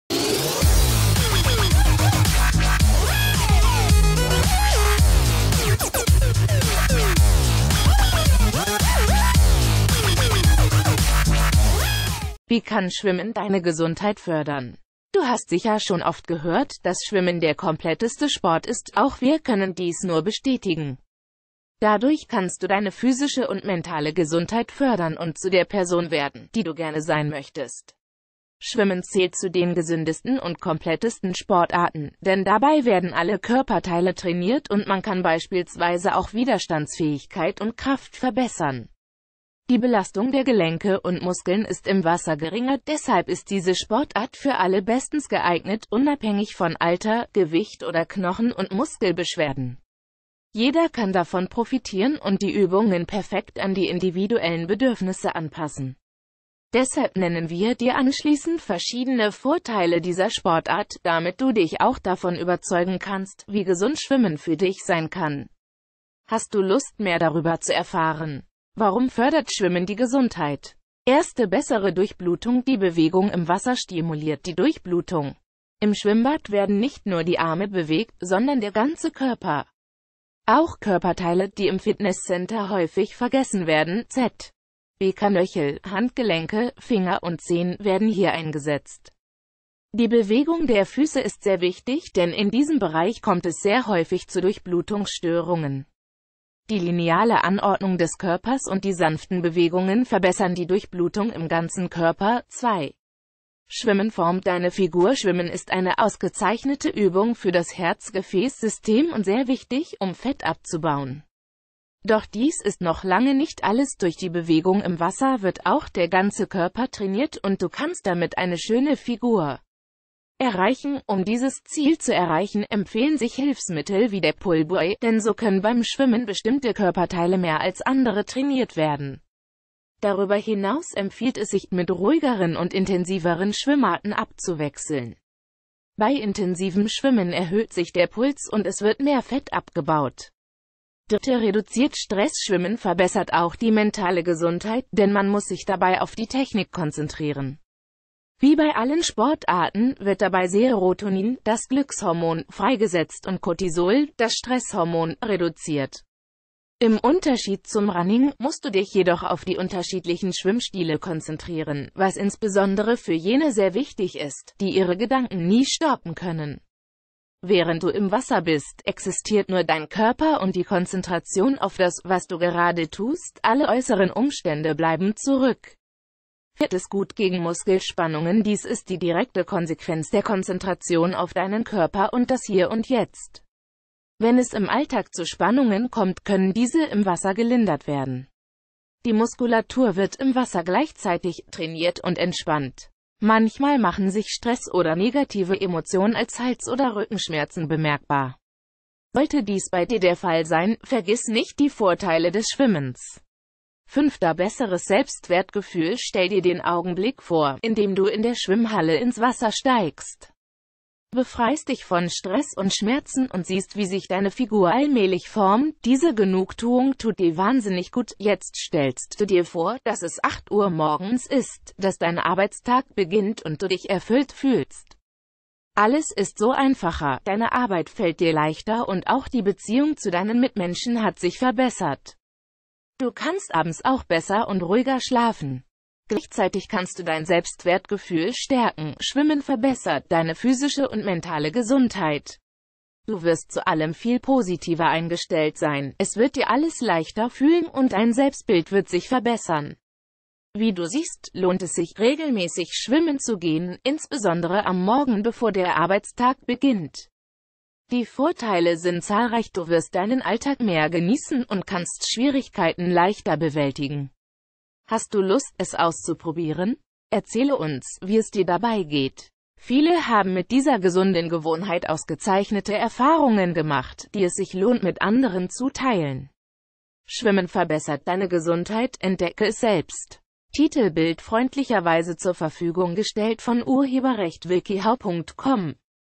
Wie kann Schwimmen deine Gesundheit fördern?
Wie kann Schwimmen deine Gesundheit fördern? Du hast sicher schon oft gehört, dass Schwimmen der kompletteste Sport ist, auch wir können dies nur bestätigen. Dadurch kannst du deine physische und ment...